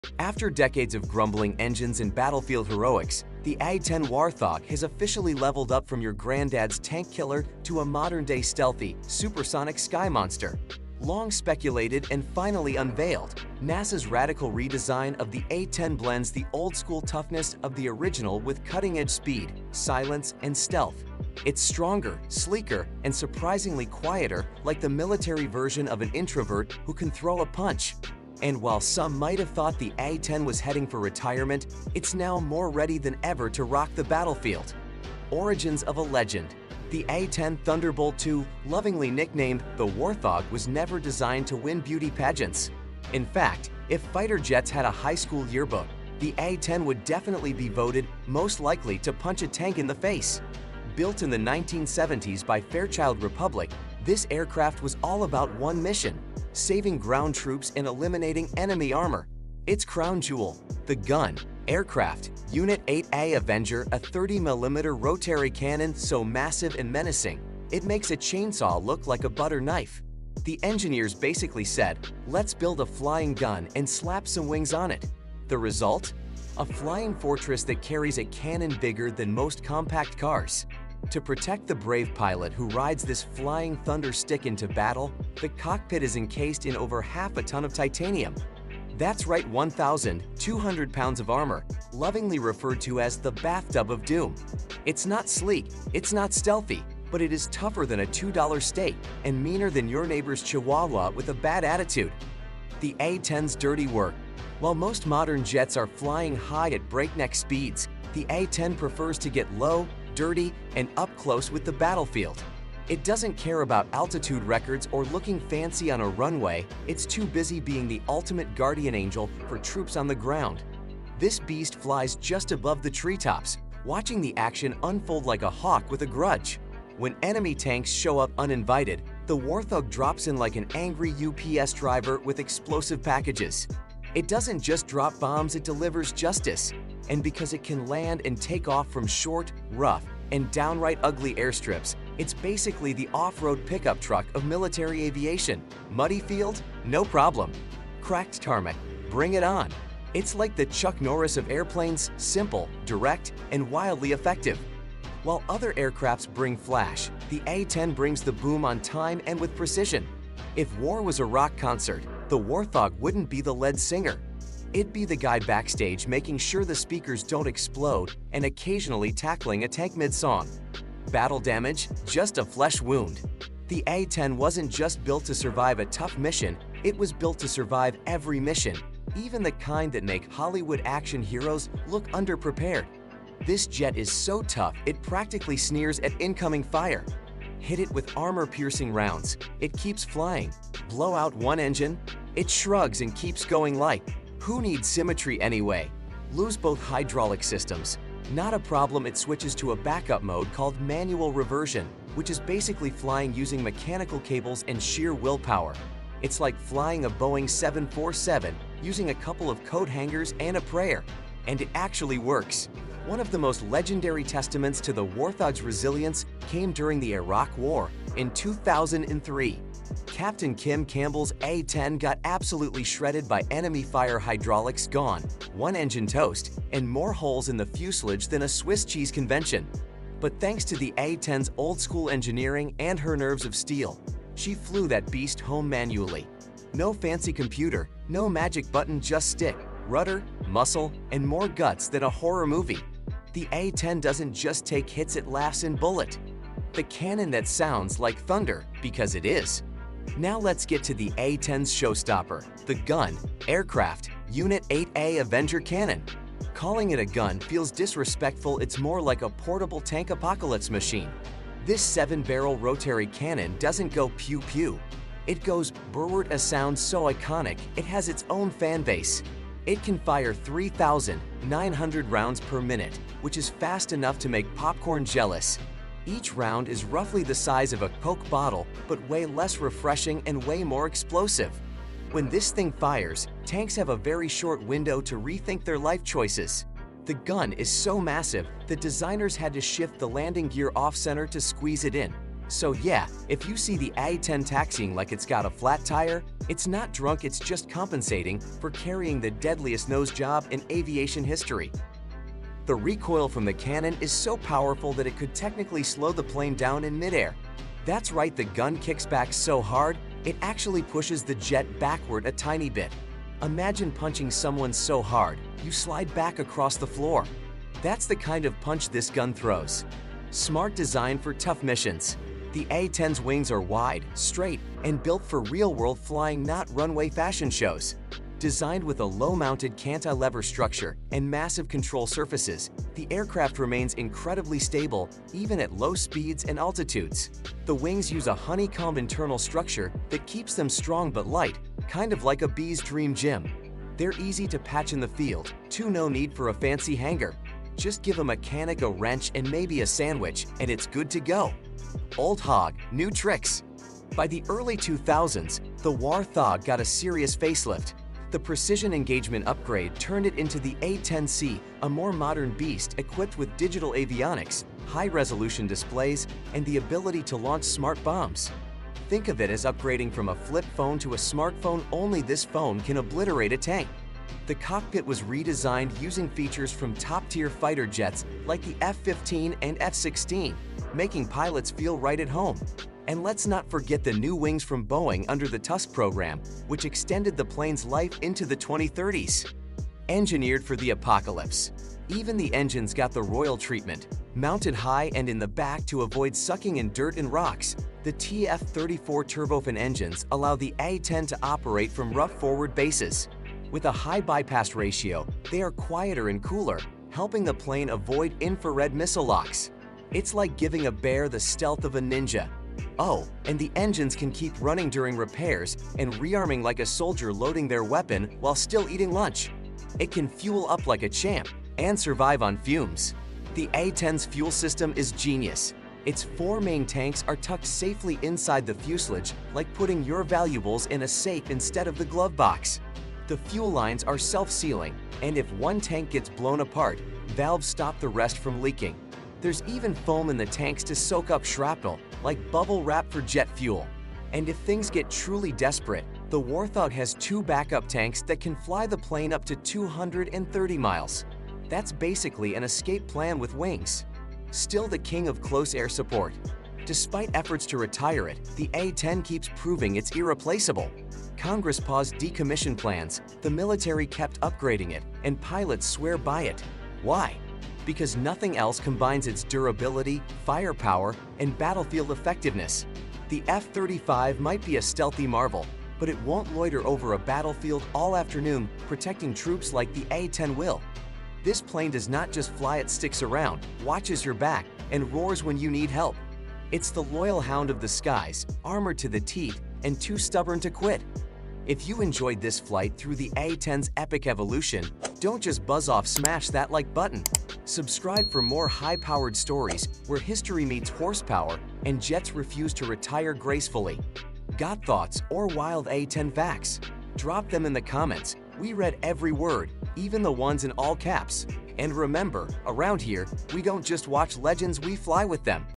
NASA Unveils Quiet Supersonic A-10 Warthog — The Beast Just Got Stealthy
Just when you thought the A-10 Warthog was heading for retirement, NASA drops a bombshell—literally. The iconic tank-buster has been reborn: sleeker, stealthier, and now, shockingly quiet. In this video, we explore the jaw-dropping evolution of the A-10 from battlefield bruiser to a next-gen sky monster. With supersonic upgrades, stealth coatings, and quieter engines, this is no longer just a flying gun—it’s a whispering warrior ready for 21st-century warfare.

We dive into the Warthog’s legendary toughness, its thunderous GAU-8/A cannon, and why even today’s most advanced jets can’t quite fill its boots in close air support. Whether you’re a long-time A-10 fan or just aviation-curious, buckle up—this isn’t your granddad’s Warthog.